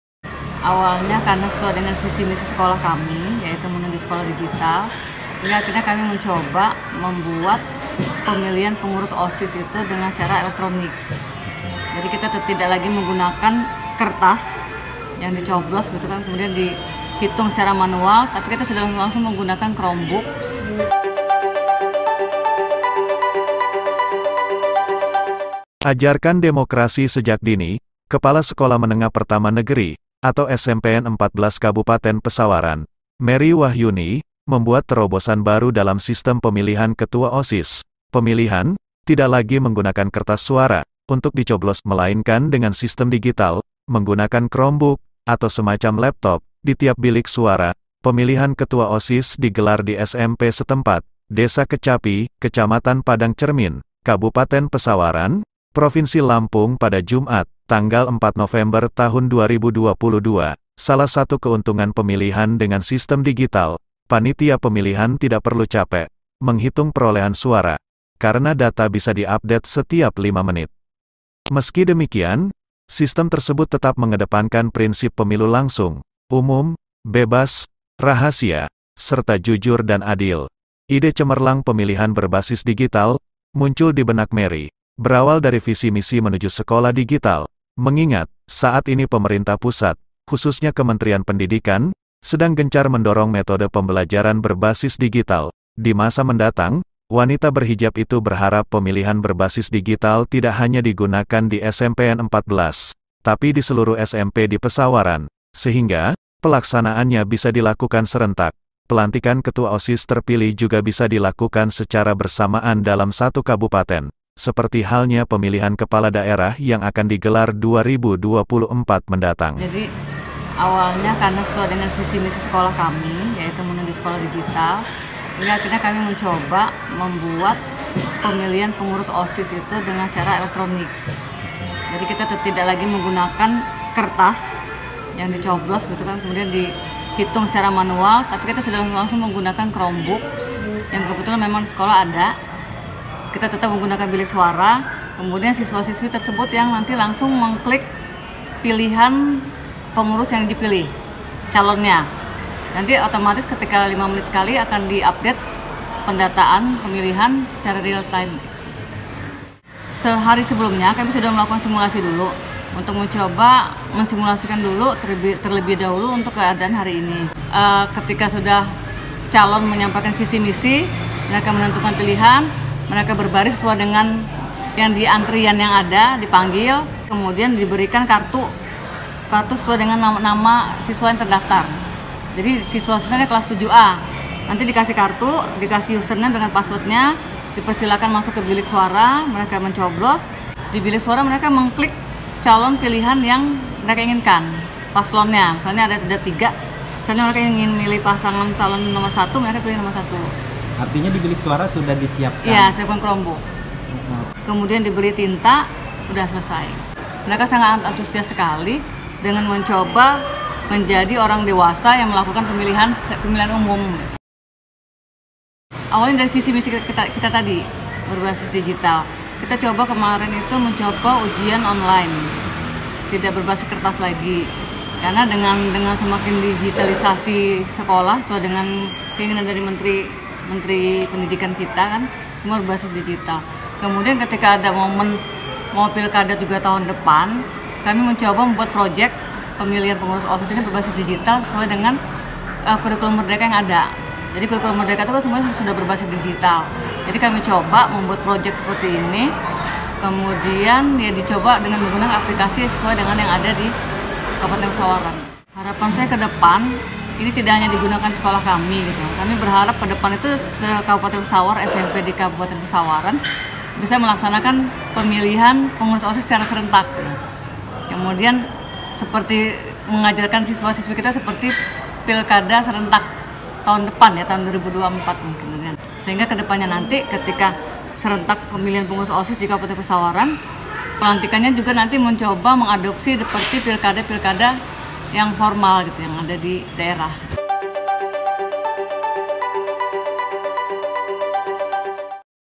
Gebrakan Baru, Kepsek di Pesawaran Gelar Pemilihan Berbasis Digital
Ajarkan demokrasi sejak dini, Kepala Sekolah Menengah Pertama Negeri, atau SMPN 14 Kabupaten Pesawaran, Merry Wahyuni, membuat terobosan baru dalam sistem pemilihan Ketua Osis.

Pemilihan, tidak lagi menggunakan kertas suara, untuk dicoblos. Melainkan dengan sistem digital, menggunakan chromebook, atau semacam laptop, di tiap bilik suara.

Pemilihan Ketua Osis digelar di SMP setempat, Desa Kecapi, Kecamatan Padang Cermin, Kabupaten Pesawaran, Provinsi Lampung pada Jumat (4/11/2022).

Salah satu keuntungan pemilihan dengan sistem digital, panitia pemilihan tidak perlu capek, menghitung perolehan suara. Karena data bisa di update setiap lima menit.

Meski demikian, sistem tersebut tetap mengedepankan prinsip luber atau langsung, umum, bebas, dan rahasia serta jurdil atau jujur dan adil.

Ide cemerlang pemilihan berbasis digital, muncul dibenak Merry, berawal dari visi-misi menuju sekolah digital. Mengingat, saat ini pemerintah pusat, khususnya Kementerian Pendidikan, sedang gencar mendorong metode pembelajaran berbasis digital.

Di masa mendatang, wanita berhijab itu berharap pemilihan berbagai digital tidak hanya digunakan di SMPN 14, tapi di seluruh SMP di Pesawaran.

Sehingga, pelaksanaannya bisa dilakukan serentak, pelantikan Ketua Osis terpilih juga bisa dilakukan secara bersamaan dalam satu kabupaten, seperti halnya pemilihan kepala daerah yang akan digelar 2024 mendatang.